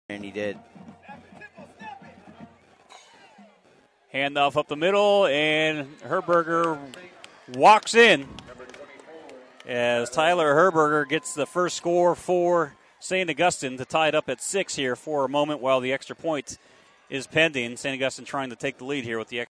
Ryan Herburger plows in for the TD
St. Augustine Ryan Herburger plows in for the 1-yard touchdown against Steele Canyon in a San Diego Section football contest.